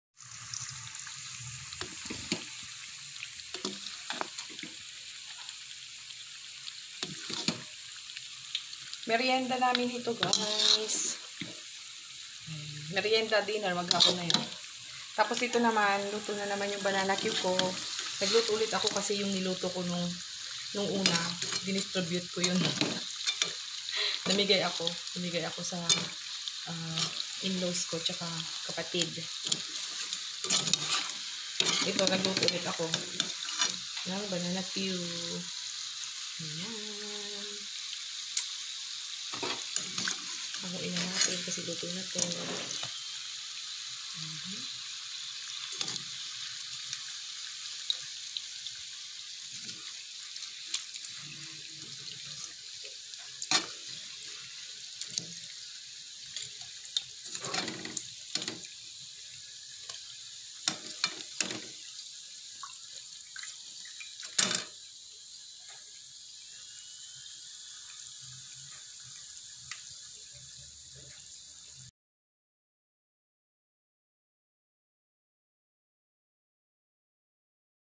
ANONG MERYENDA NG KULONG BAHAY LANG || BUHAY PROBINSIYA
Hi guys in this video..i will show you on what kind of food i cook for our "Meryenda  ng kulong bahay lang"Buhay probinsiya lutong Ilokano..

MERYENDA
SAGING
BANANA QUE
PANCIT
PANSIT GUISADO
PANSIT GUISADO
LUTONG ILOKANA
PANLASANG PINOY
BUHAY PROBINSIYA